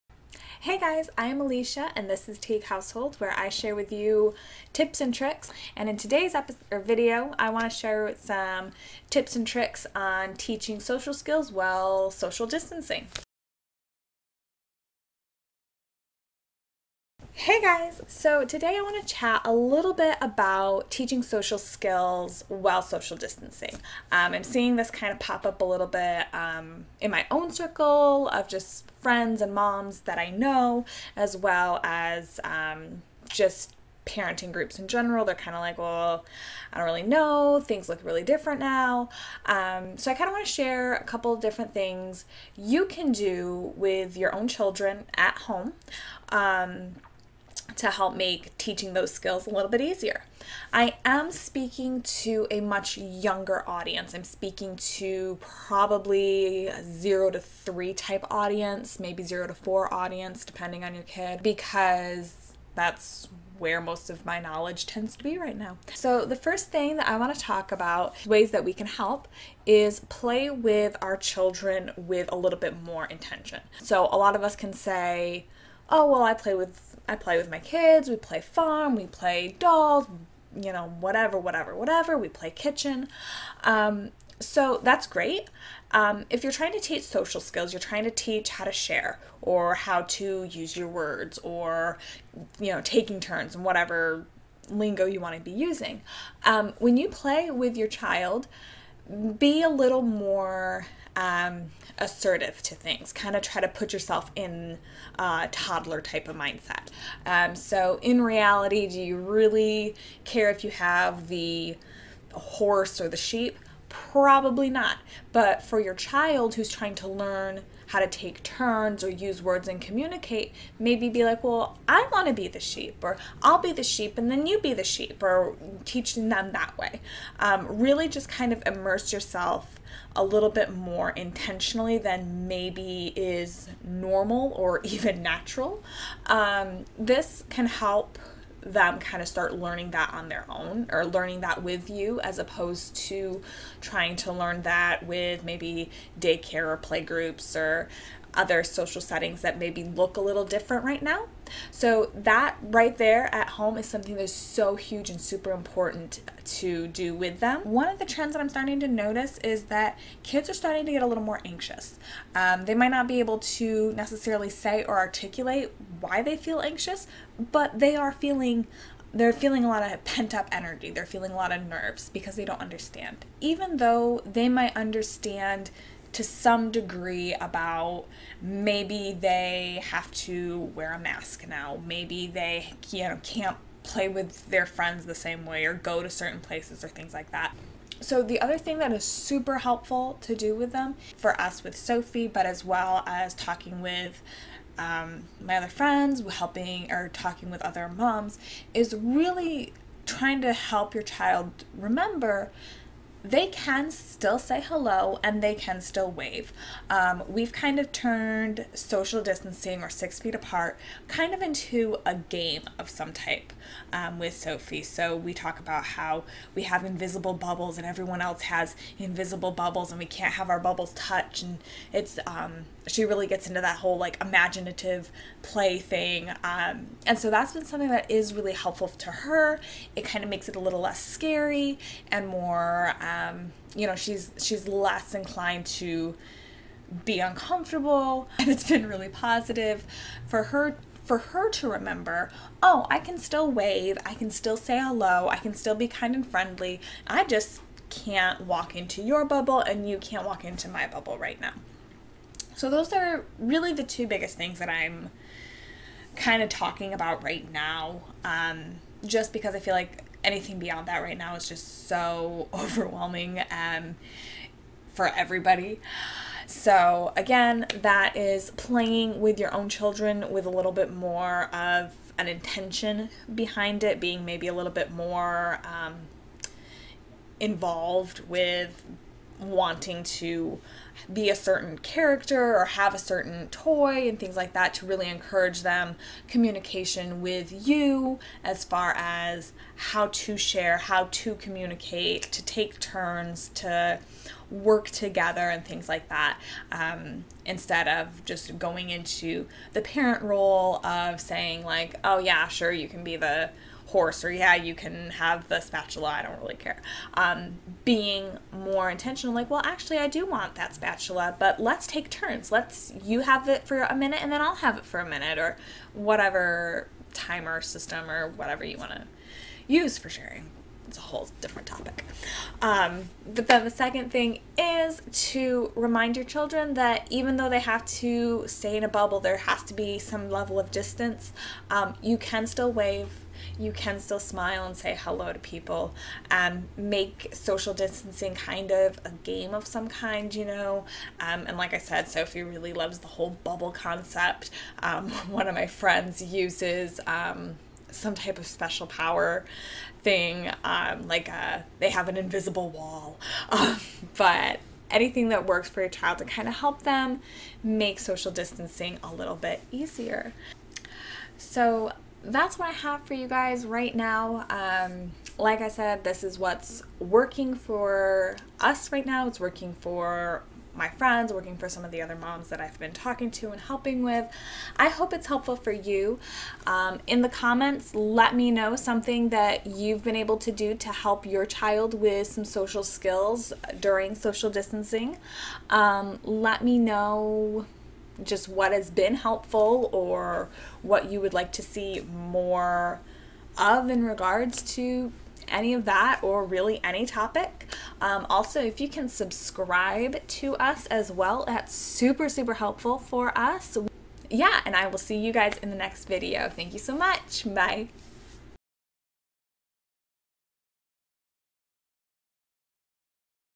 Toddler Social Skills While Social Distancing
How can you help your toddler develop social skills while social distancing? This video covers 2 easy and effective ways to promote social skills in your toddler EVEN while quarantined and social distancing.

Please share tips of your own!